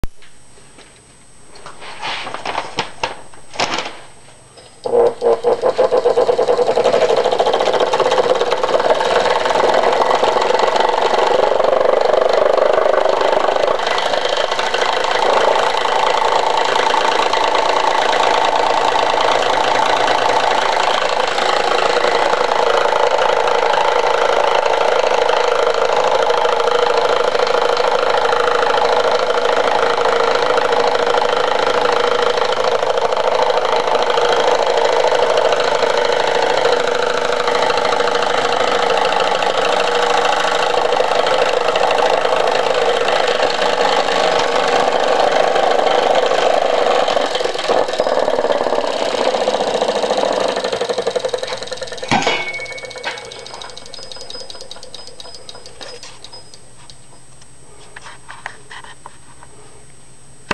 Electric Engine 120 Volt AC 1600 RPM
Electric Engine.  This is how the first electric motors were built in the late 1800s.  The engine turns 1600 RPMs.